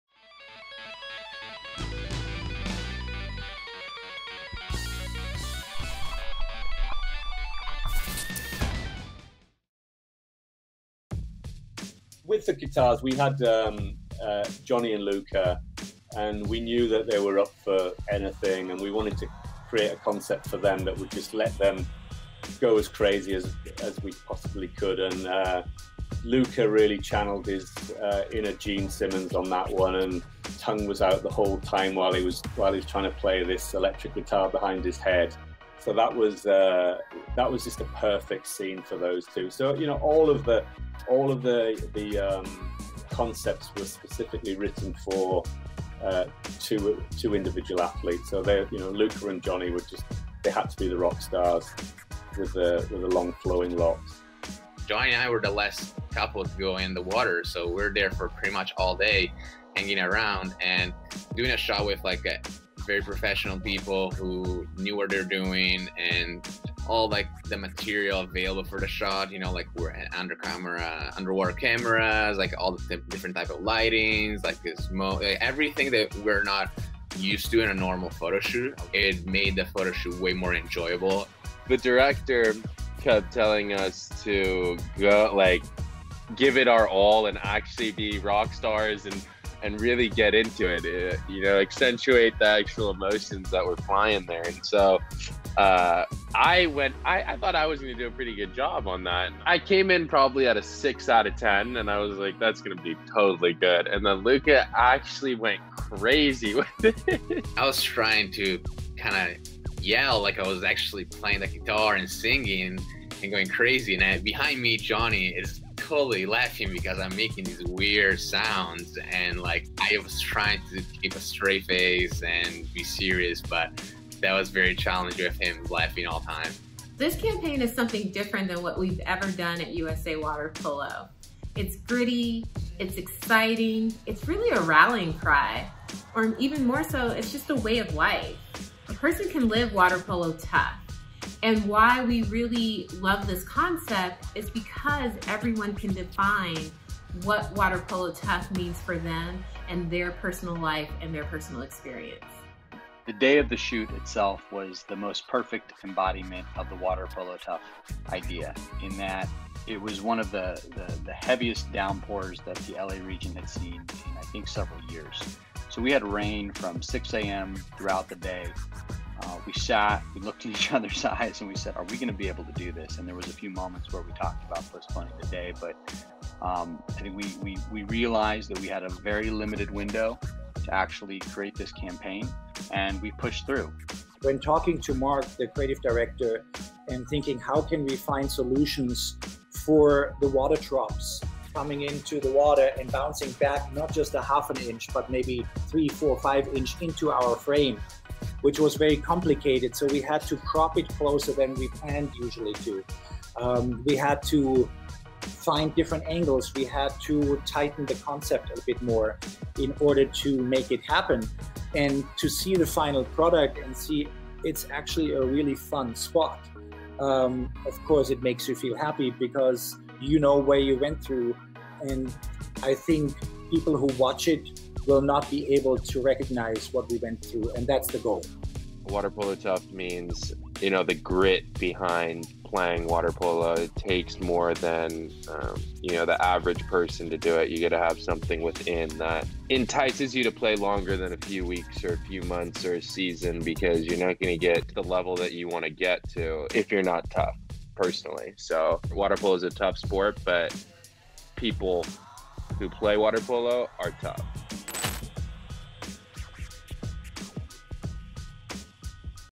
Water Polo Tough: Behind The Scenes of Shred
The Athletes and creatives involved in the making of "Shred" give you a behind the scenes look at this electric video and why they enjoyed creating this piece for the Water Polo Tough campaign.